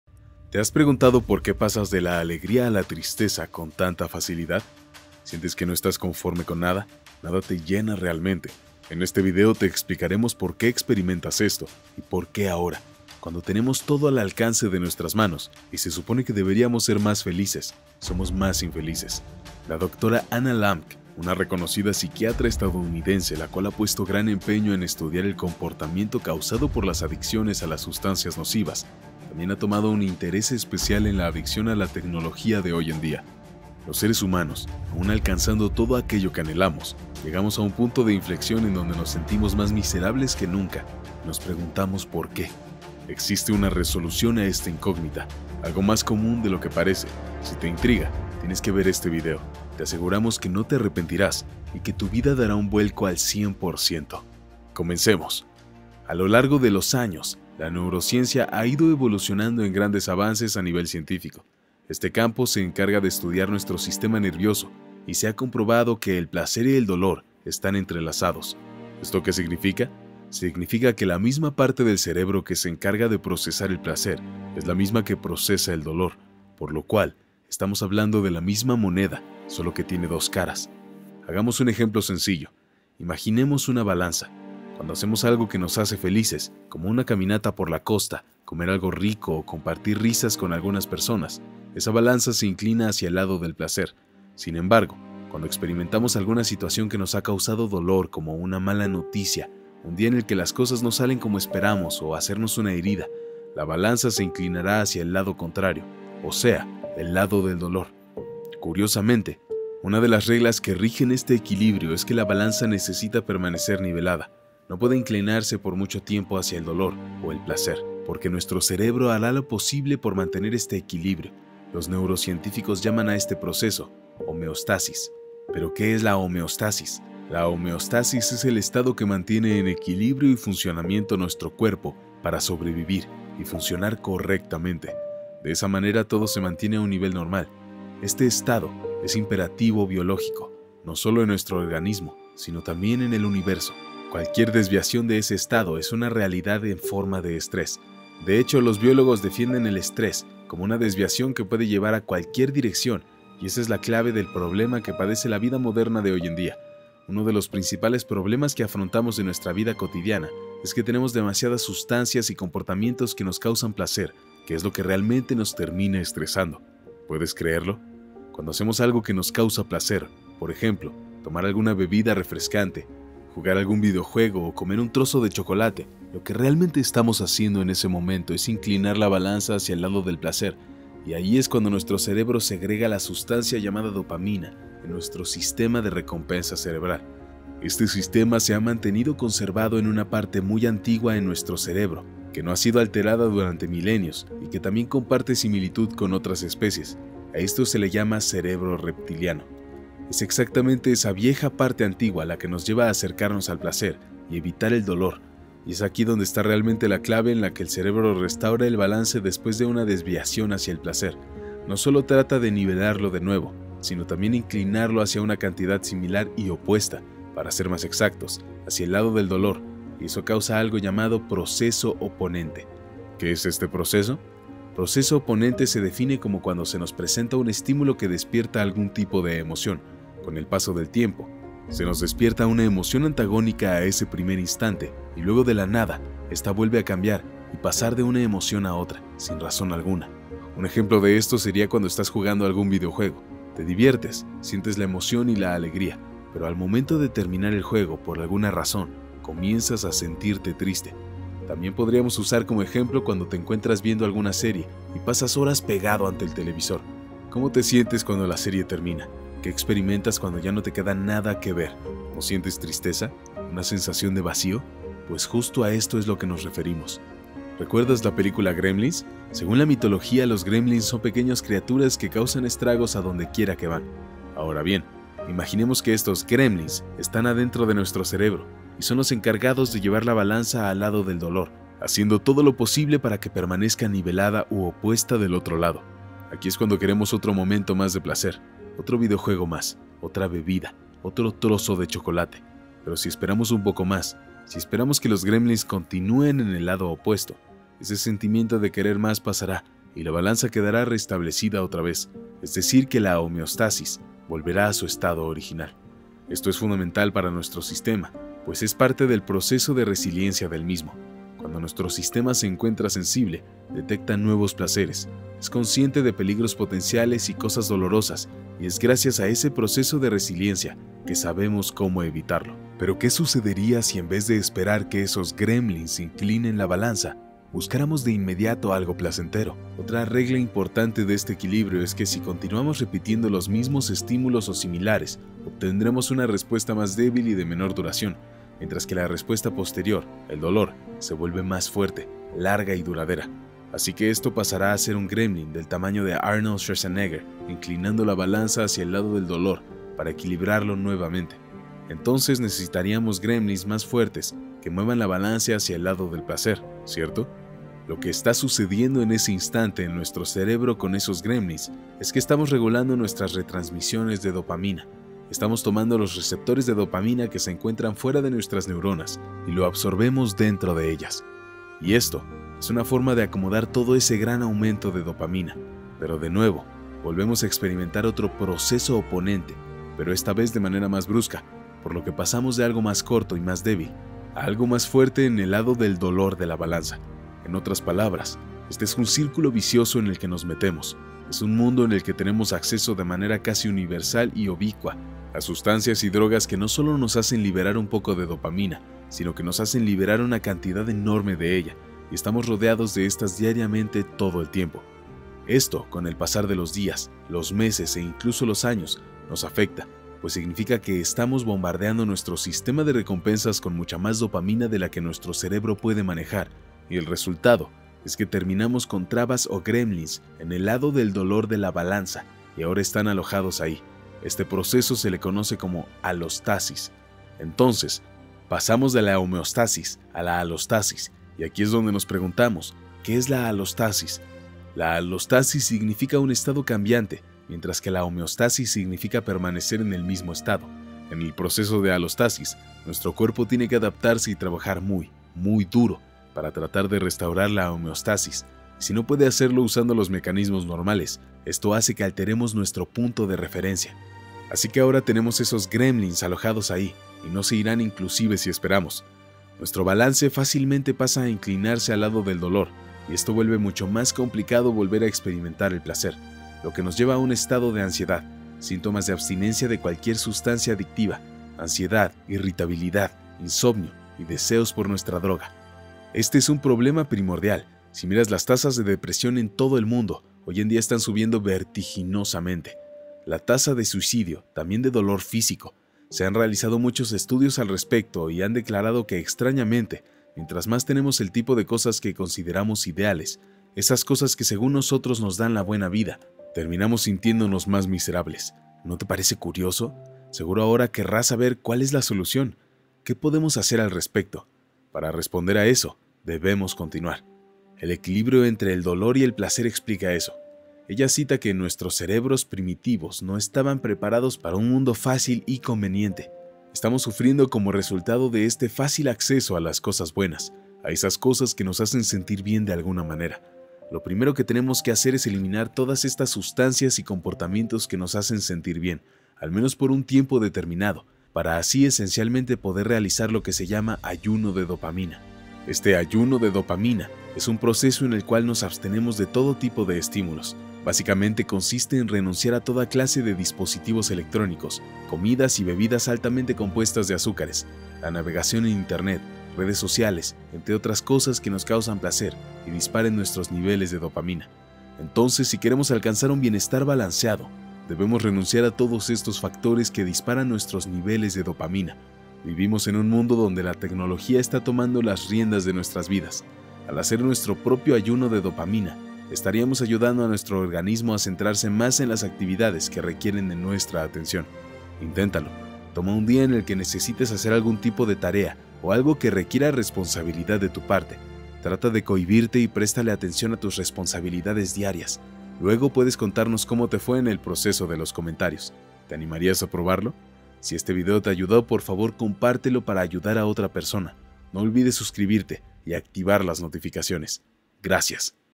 "Tu comportamiento se REINICIARÁ al 100%" ¡Esto es como tener un SUPERPODER! - ¡Úselo AHORA!
✅ Descubre los SECRETOS más IMPORTANTES para superar la ADICCIÓN a la tecnología y así recuperar TU VIDA

🥇 ¿Te has preguntado por qué pasas de la alegría a la tristeza con tanta facilidad?¿Sientes que no estás conforme con nada?  ¿Nada te llena realmente? En este video te explicaremos por qué experimentas esto y por qué ahora, cuando tenemos todo al alcance de nuestras manos y se supone que deberíamos ser más felices, somos más infelices.

La Dra. Anna Lembke, una reconocida psiquiatra estadounidense la cual ha puesto gran empeño en estudiar el comportamiento causado por las adicciones a las sustancias nocivas, también ha tomado un interés especial en la adicción a la tecnología de hoy en día.
Los seres humanos, aun alcanzando todo aquello que anhelamos, llegamos a un punto de inflexión en donde nos sentimos más miserables que nunca y nos preguntamos ¿Por qué?

Existe una resolución a esta incógnita, algo más común de lo que parece. Si te intriga, tienes que ver este video.